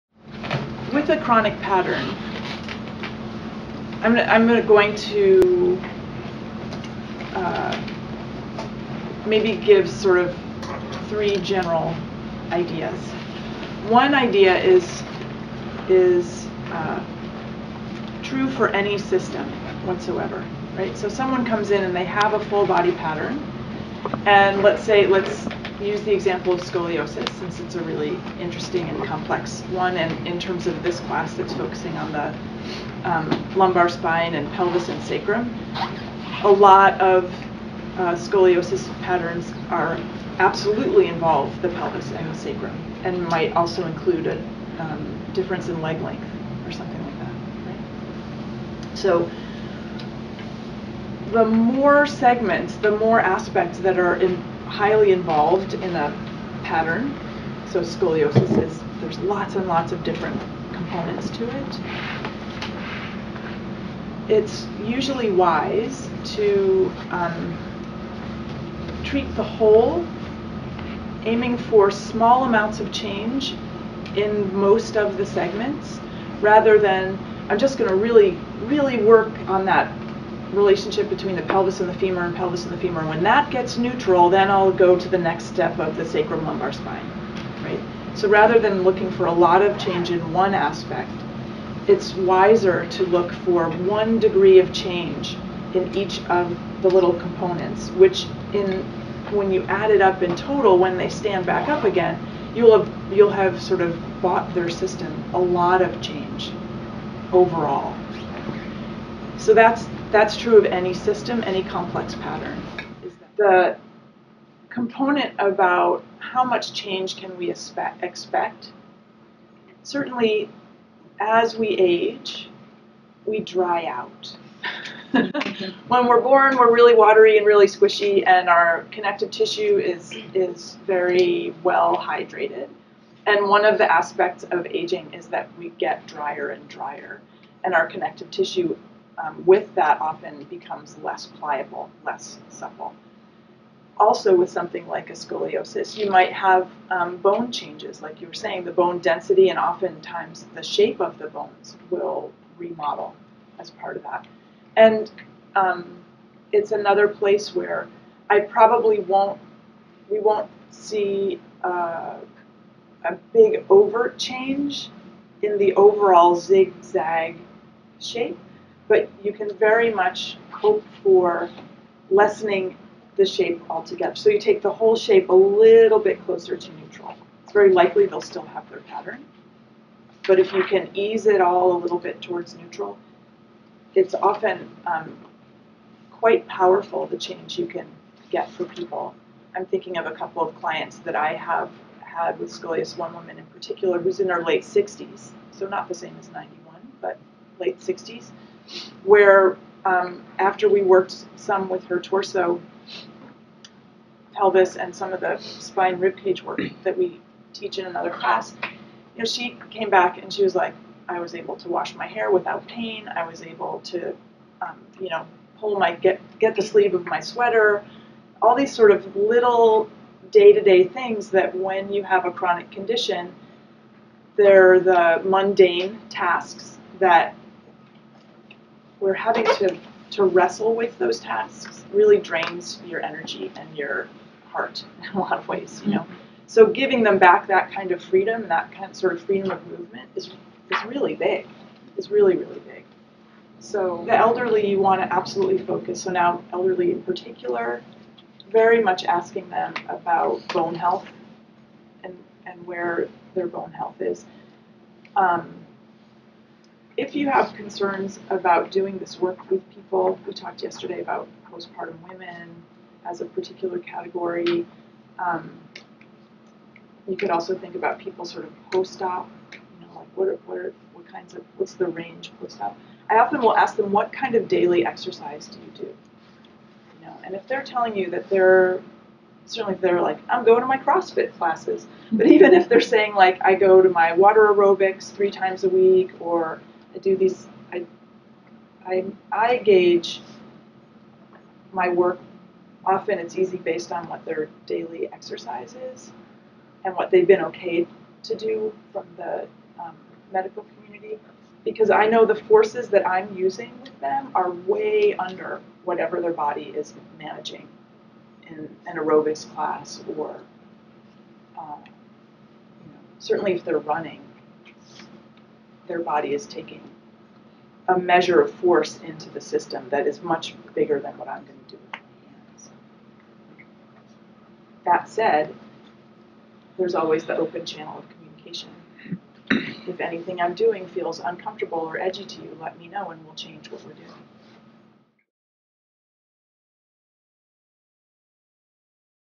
Strategies for working with full body patterns
Lauren Christman discusses strategies for changing full body patterns in our Myofascial Balancing the Pelvis, Sacrum and Low Back class.